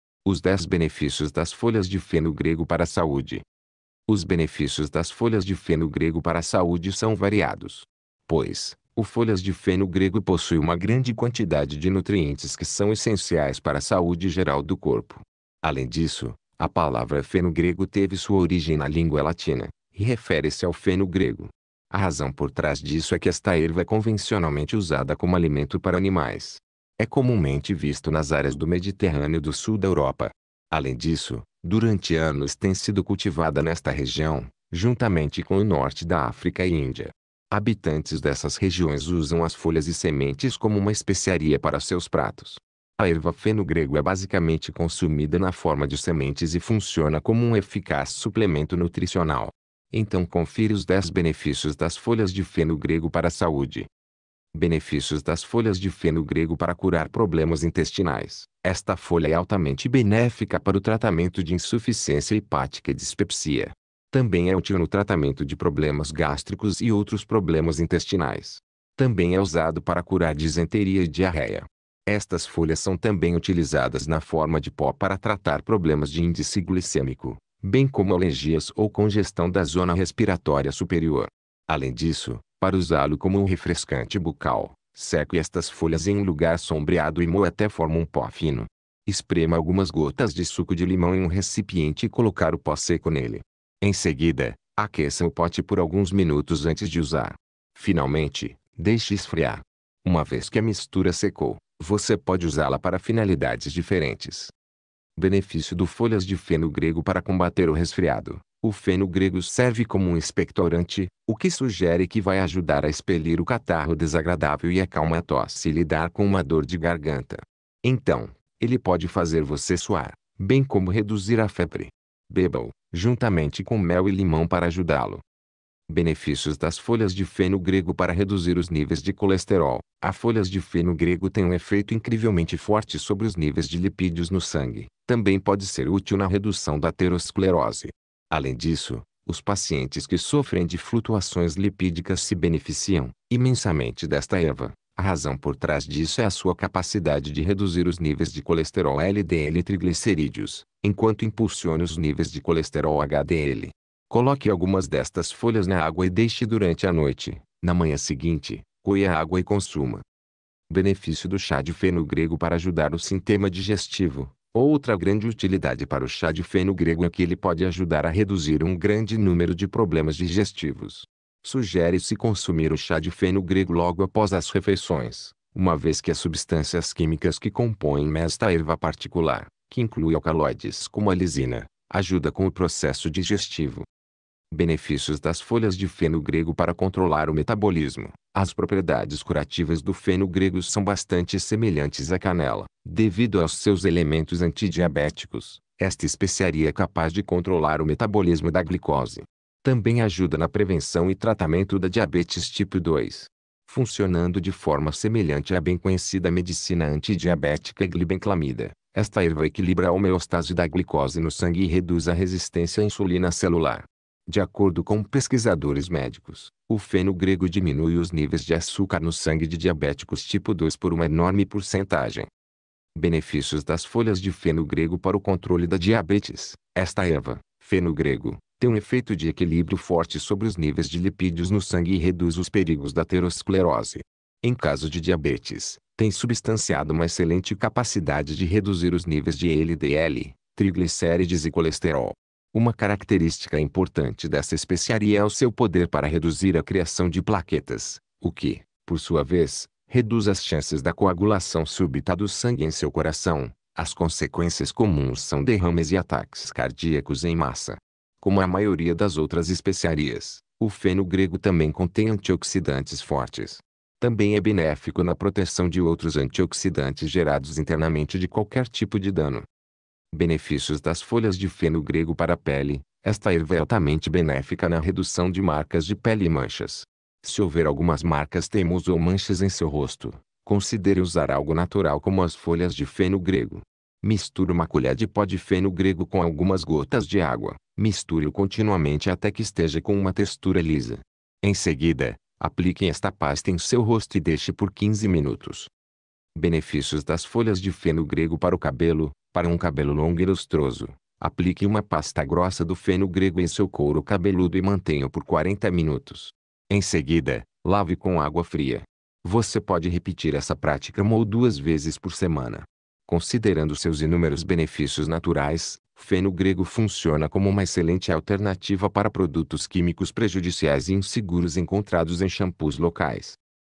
Os 15 Benefícios da Folha de Graviola Para Saúde
Os Benefícios da Folha de Graviola Para Saúde são variados, pois, a Folha de Graviola possui uma grande quantidade de nutrientes que são essenciais para saúde geral do corpo. Além disso, a graviola é uma fruta que tem sua origem nas florestas da América do Sul, África e Sudeste Asiático. Esta é uma árvore de folhas largas sempre verdes cuja cada parte é útil e tem propriedades medicinais.

Esta fruta é extremamente deliciosa com um aroma intenso e um sabor agridoce que é basicamente uma combinação do sabor do abacaxi e morango. Recentemente, ganhou atenção e popularidade devido às suas propriedades naturais de combate às células cancerígenas. Além de suas propriedades anti-câncer, tem vários outros benefícios médicos.